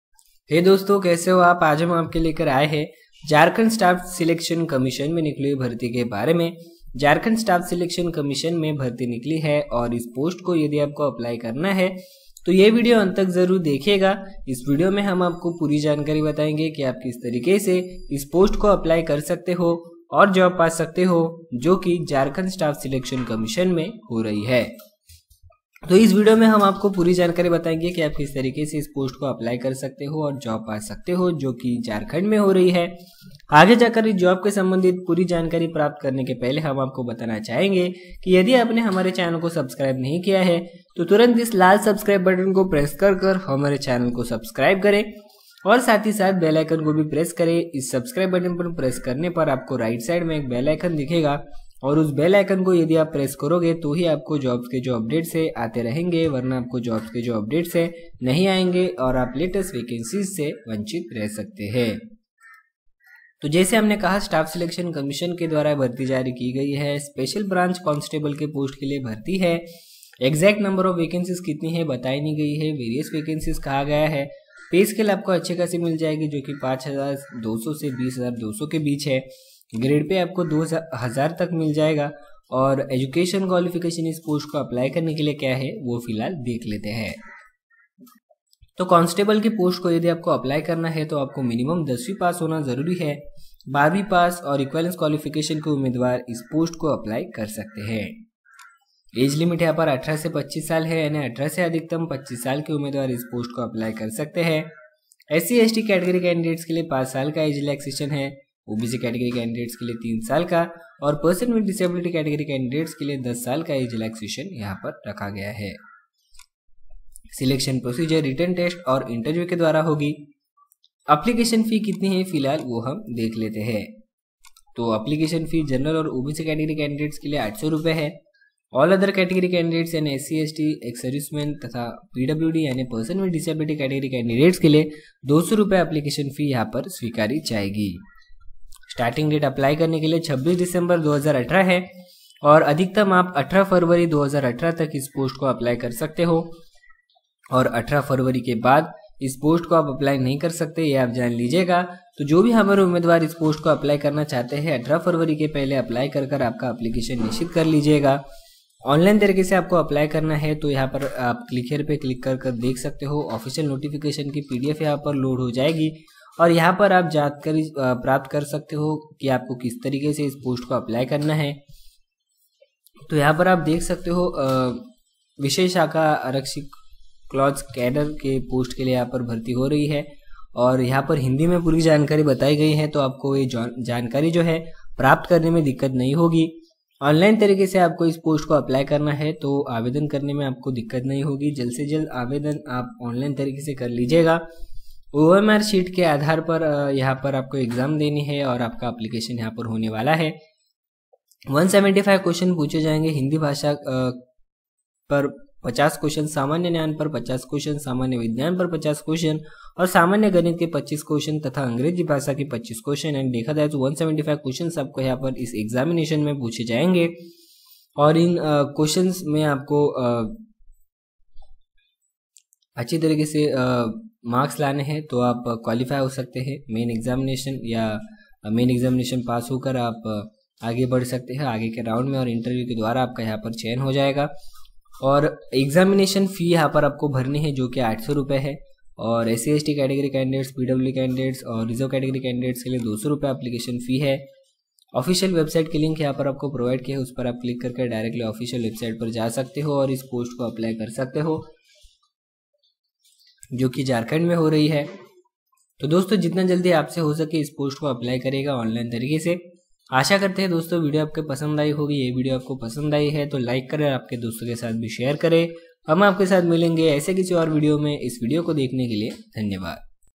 JSSC Recruitment 2019 - Jobs in February - Latest Freshers jobs - Govt Jobs
Today's job about JSSC Recruitment. The following job is on Regular basis. For more detailed updates kindly visit the link given Above. You can  apply for this job through the link provided